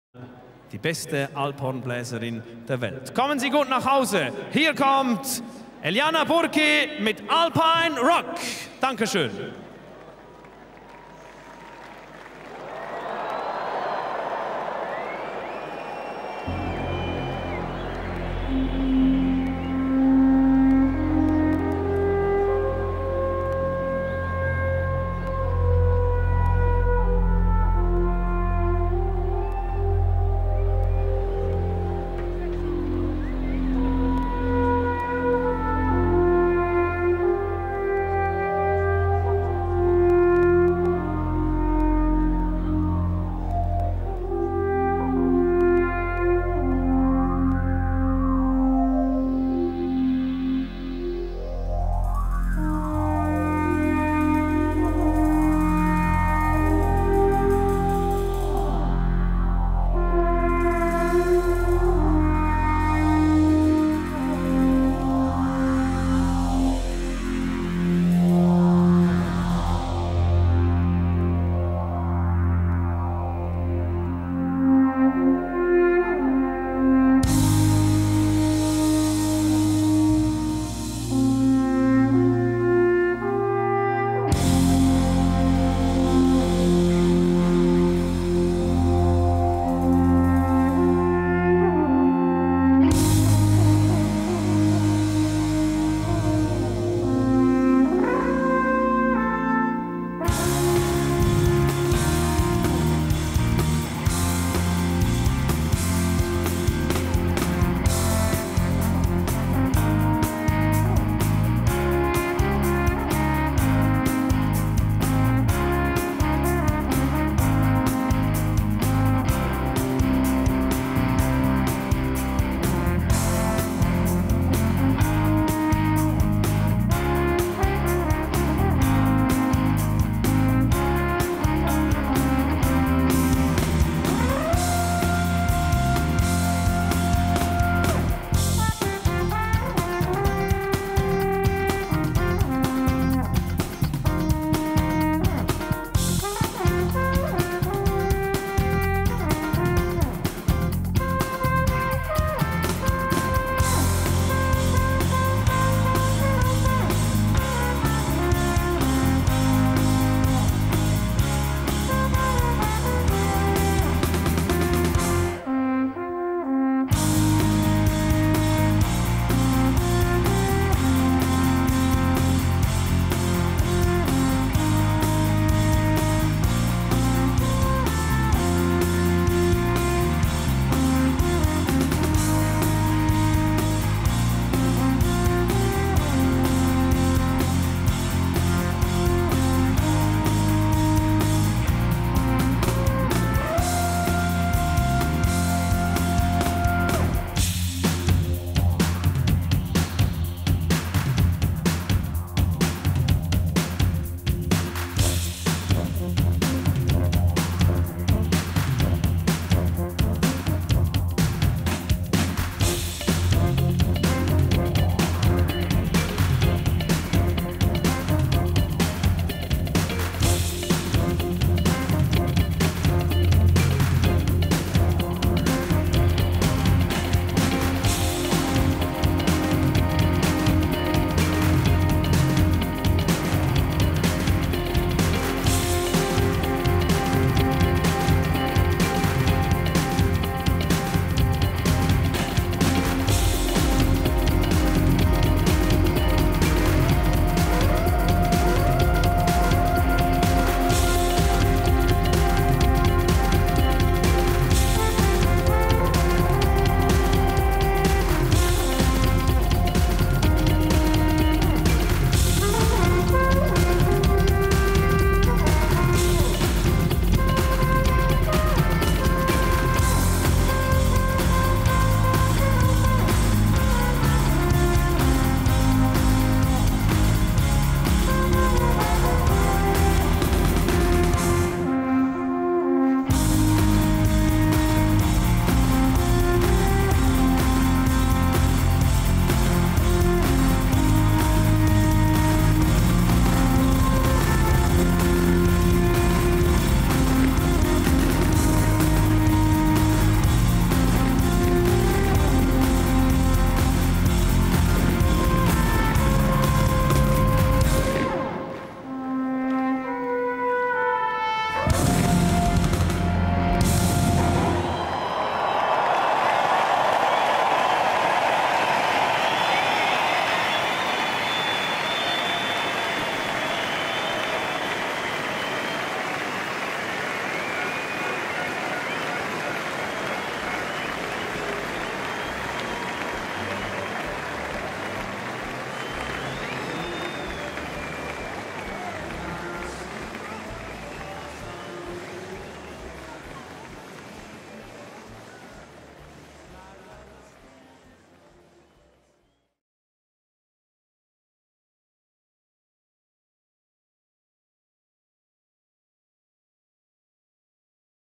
Eliana Burki - Weltklasse Zürich 2009 (Closing Ceremony)
Eliana Burki performing at the closing ceremony of Weltklasse Zürich 2009.